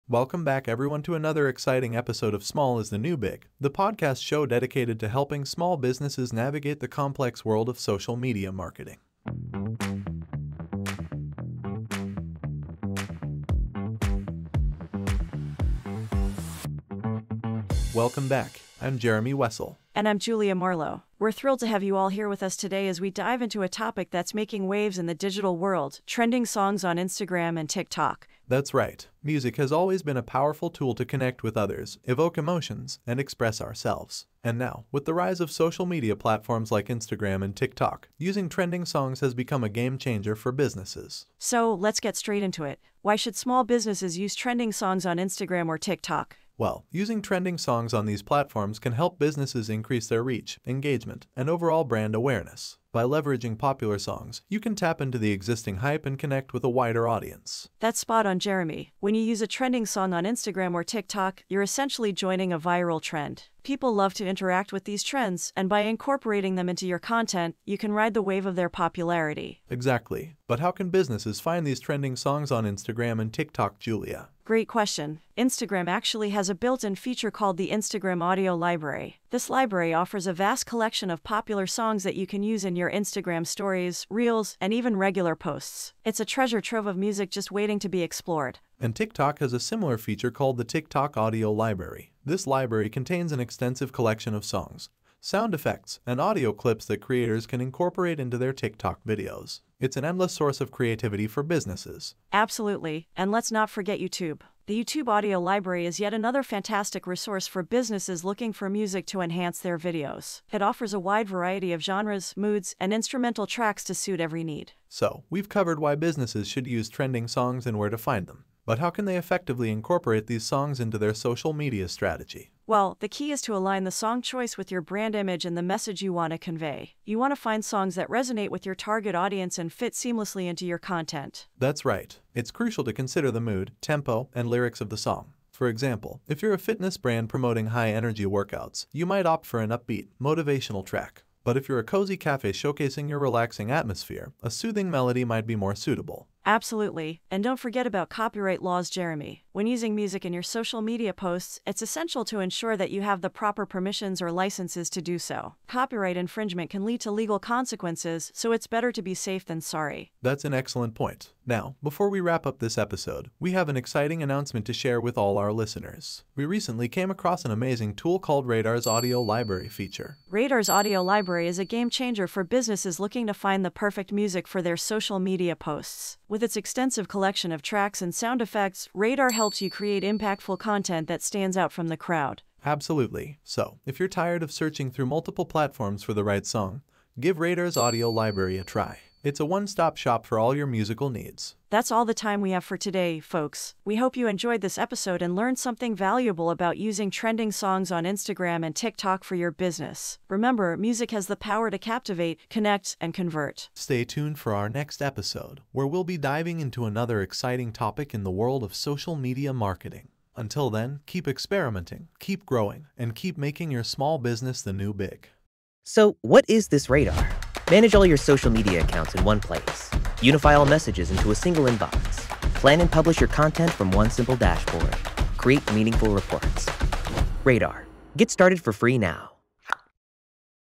How to find trending music for your posts?
Welcome to another exciting episode of Small Is The New Big! In this episode, get ready to explore all things trending songs on Instagram and TikTok – it's going to be an absolute blast!

Join Julia and Jeremy as they uncover the power of using trending songs on both Instagram and TikTok to take your business to new heights. Discover how these catchy tunes can captivate your audience and elevate your marketing strategy like never before.

But that's not all! Learn how to leverage these incredible resources to creatively enhance your content and grab attention in all the right ways.

Whether you're a small business owner, an aspiring influencer, or simply a social media enthusiast, this episode is a must-watch. So, grab your favorite beverage, sit back, and immerse yourself in the world of trending songs on Instagram and TikTok – you won't want to miss it.